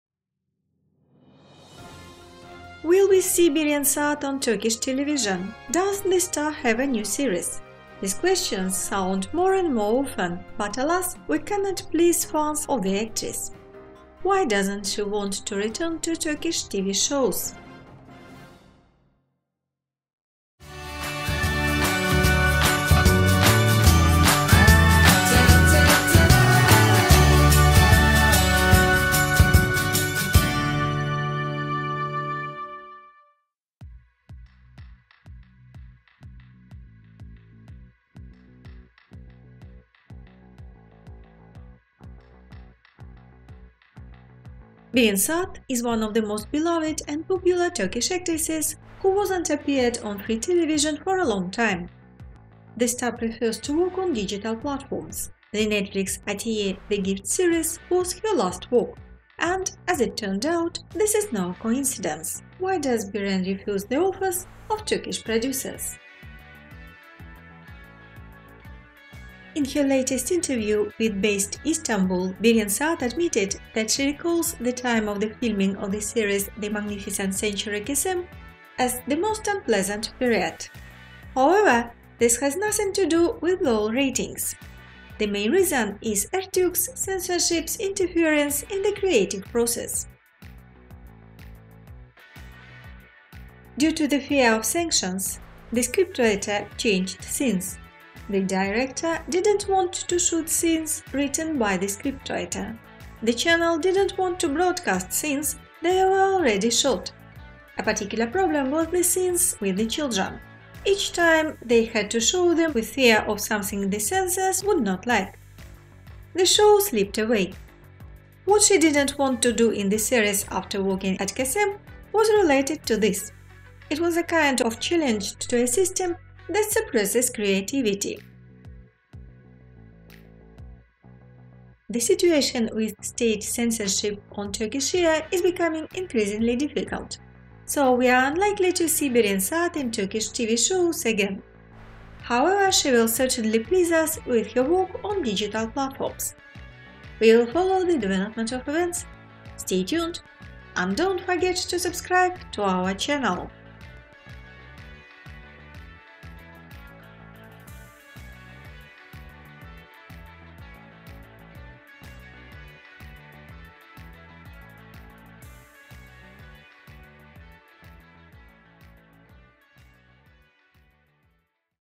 Why does Beren Saat not want to work in Turkish TV shows?
Will we see Beren Saat on Turkish television? Does the star have a new series? These questions sound more and more often, but alas, we cannot please fans of the actress. Why doesn’t she want to return to Turkish TV shows?